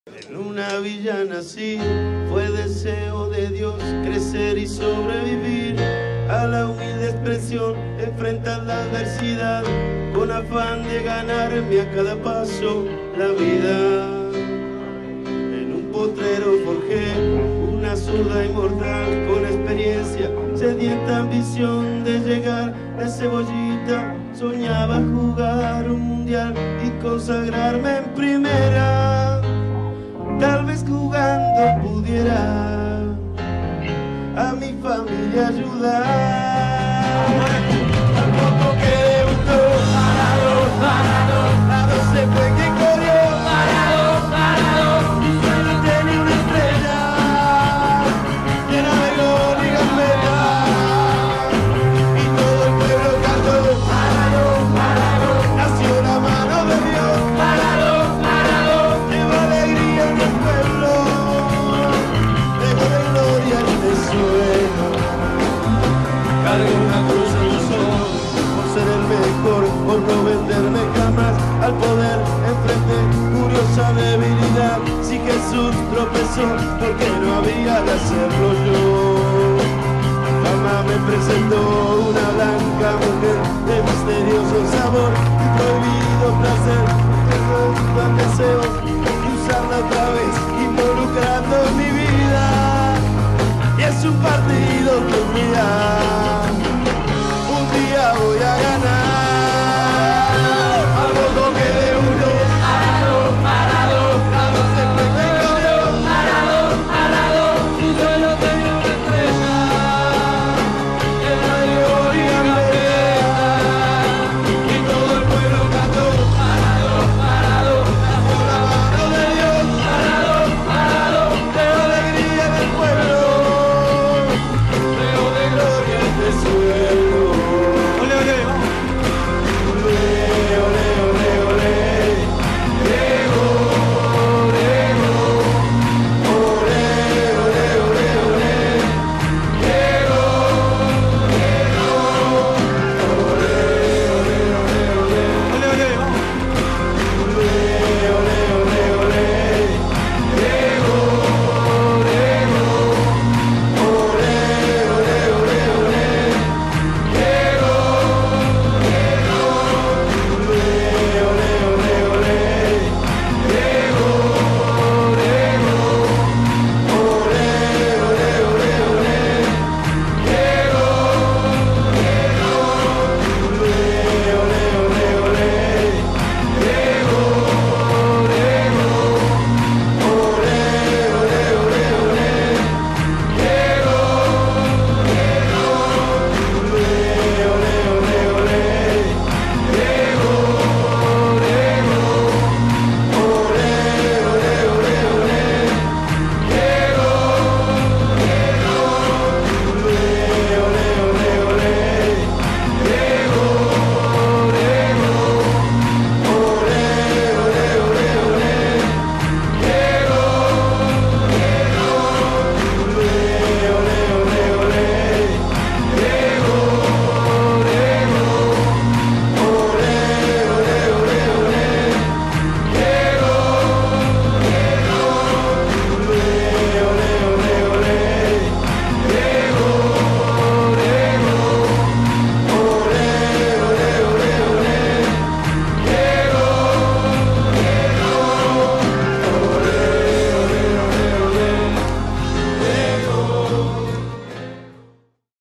Diego Armando Maradona ci lascia a 60 anni
Il mondo del calcio saluta il numero uno di tutti i tempi. Video Michele Marzullo.